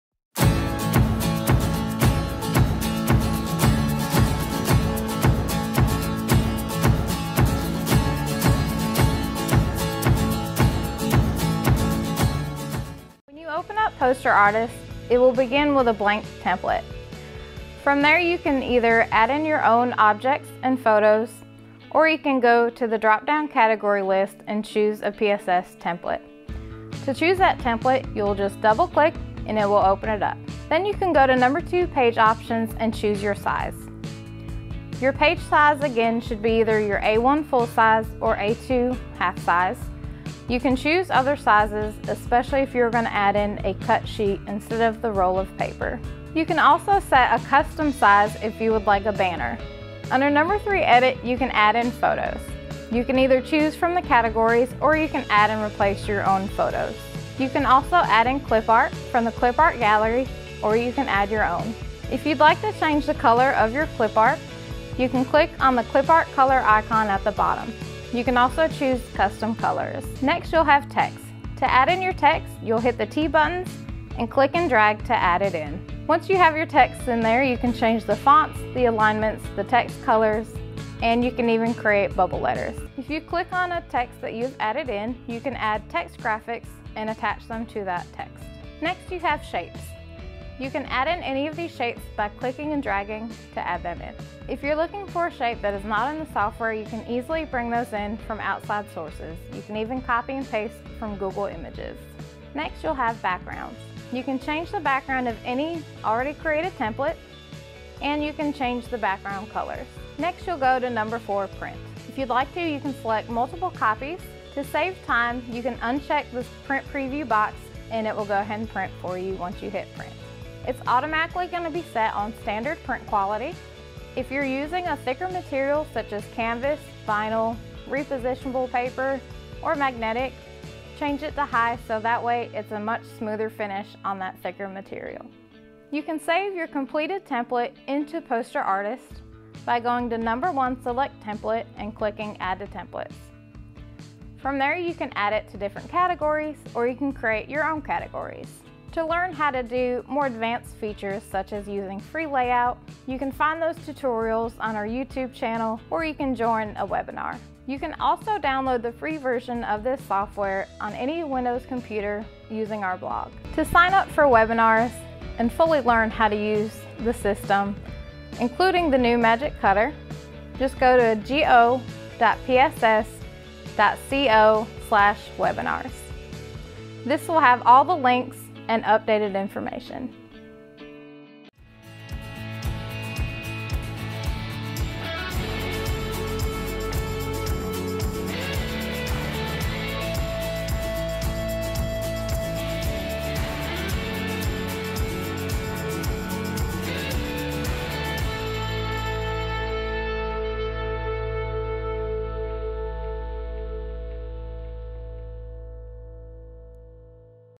LOD Poster Maker Training PosterArtist
In this video, we will train you on how to use PosterArtist to print on your Learn OnDemand Poster Maker. PSS is dedicated to collaborative student success.